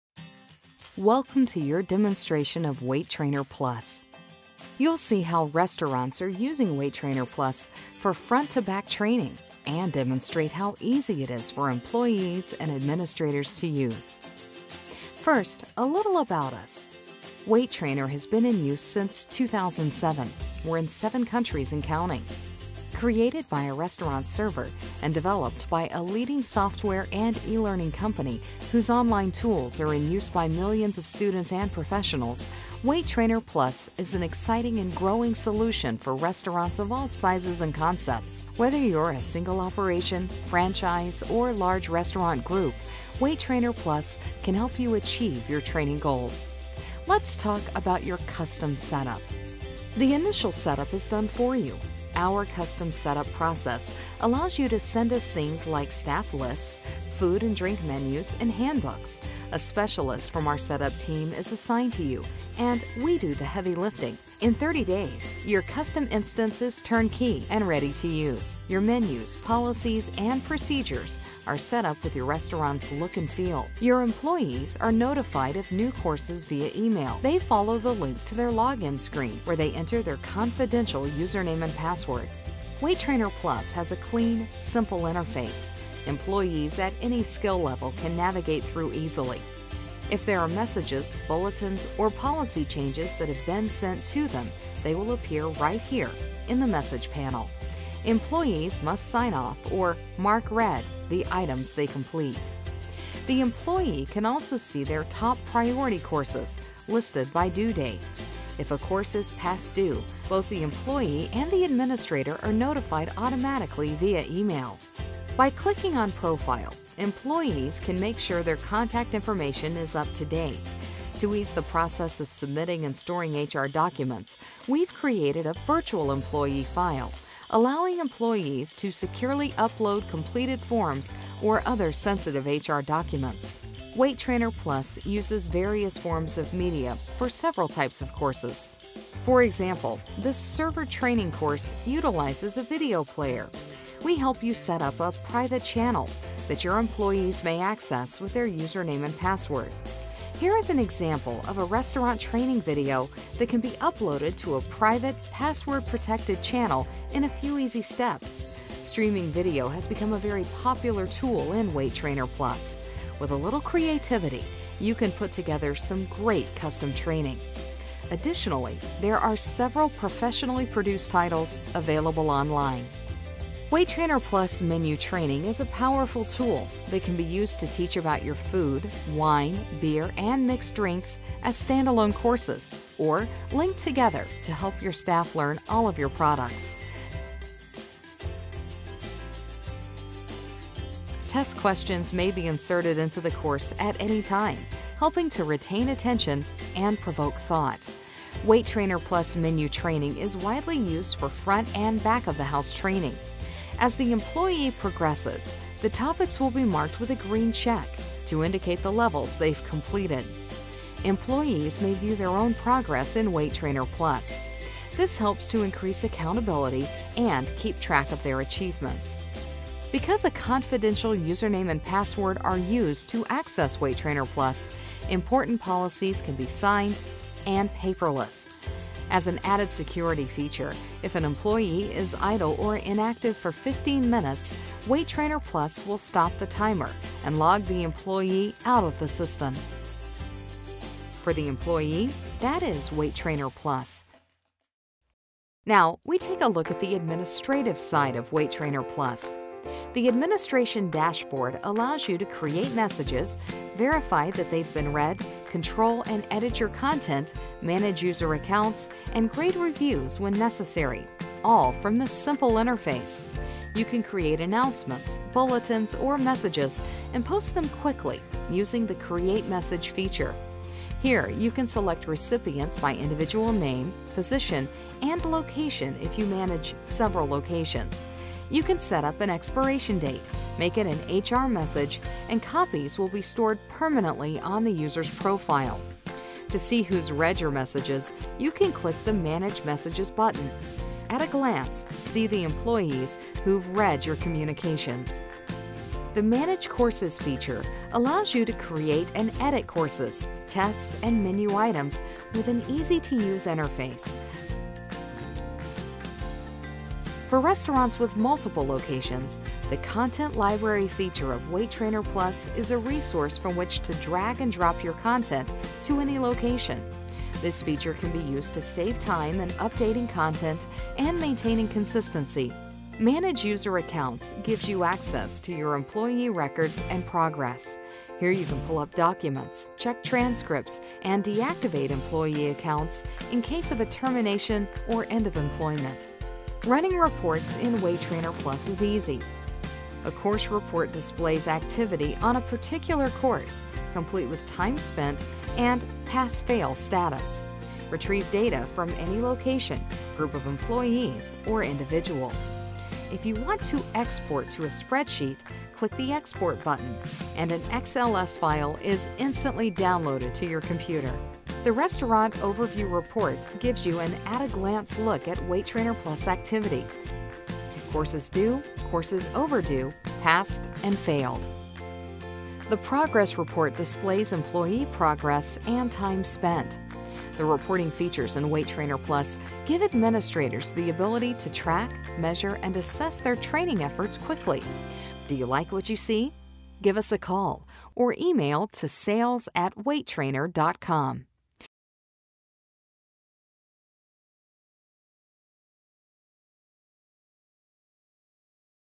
Waitrainer+™ Demo
In this video, we show you how Waitrainer+™ looks and feels from both employee and admin sides of the program.

Waitrainer+™ is the new standard in online restaurant training. Visit us at waitrainer.com for more information.